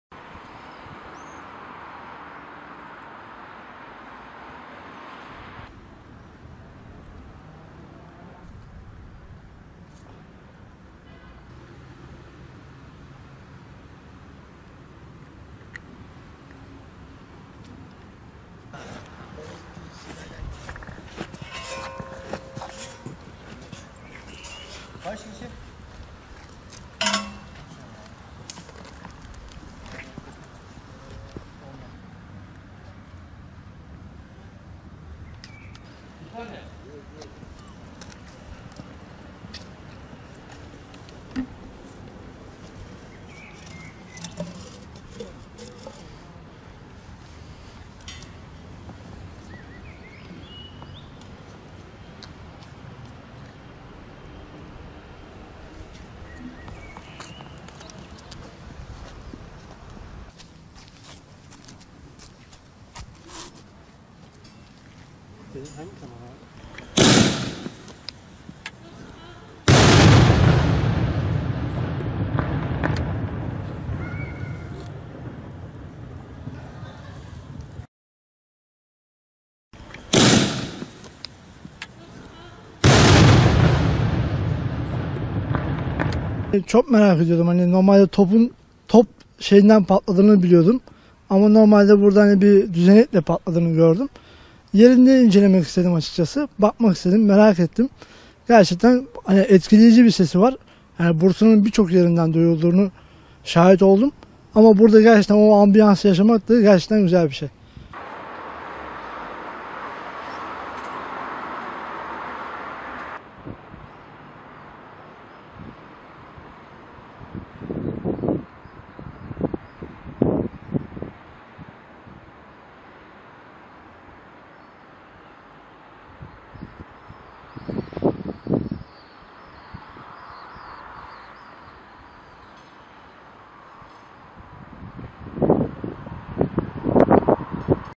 Bursa'da tophane surlarından iftar topu böyle görüntülendi!
Evliyalar şehri Bursa'da ramazan ayında iftar zamanı patlayan ve tüm kentte yankılanan herkesin merak ettiği düzenek böyle görüntülendi.